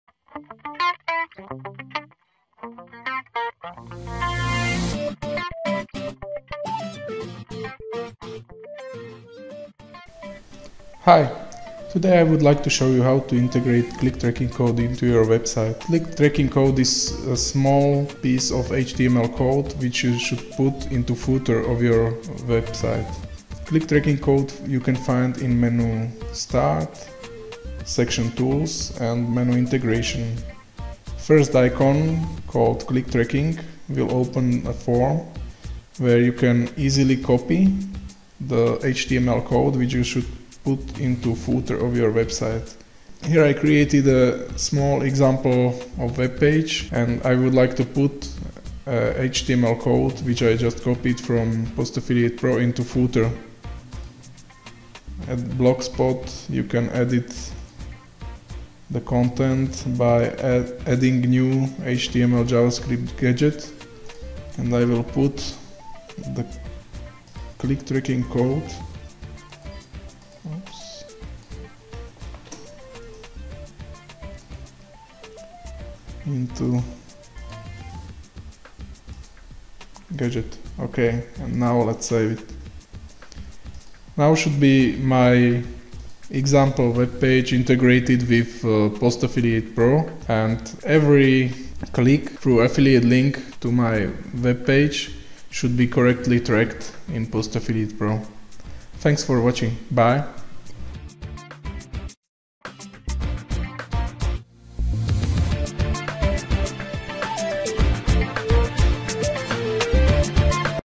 Click Tracking Code Integration - Affiliate Software
- Short tutorial how to add affiliate software click tracking code into your website.